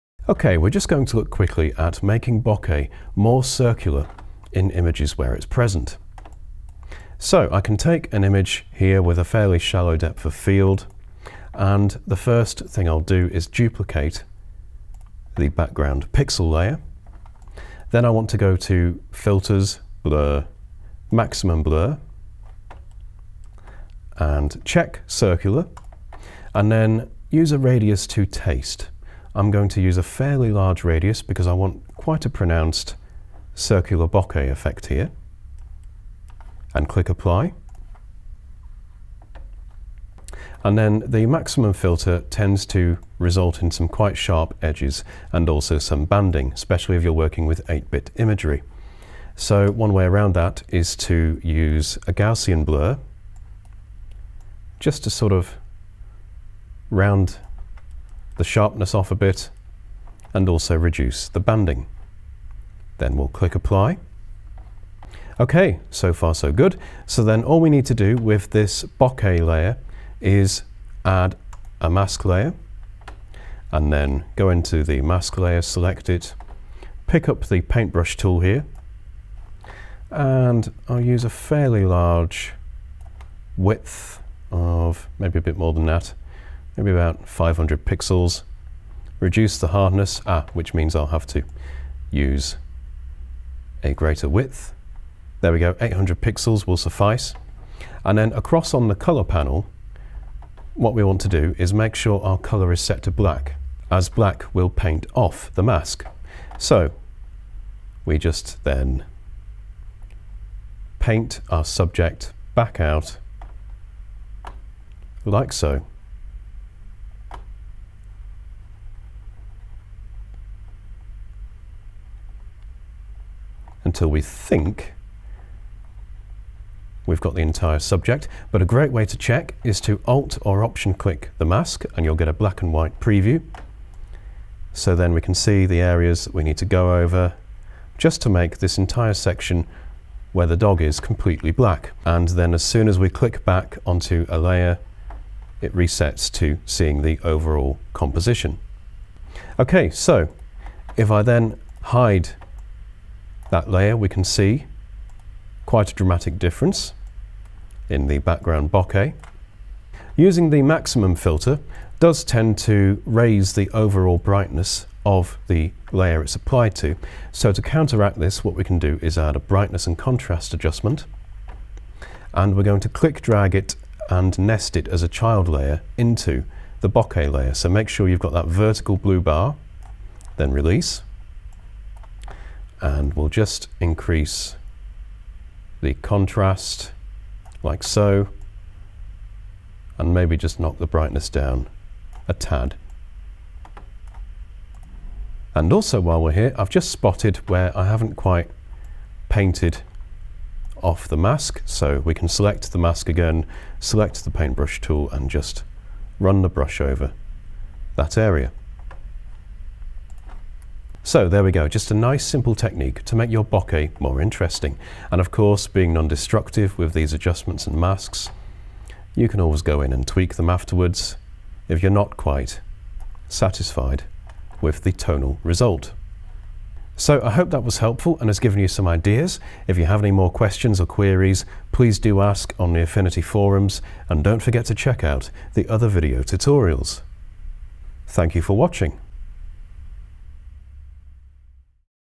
Circular Bokeh (Affinity Photo)
See how to turn boring or confusing bokeh into more appealing circular bokeh!

Credits: Photograph by James Ritson.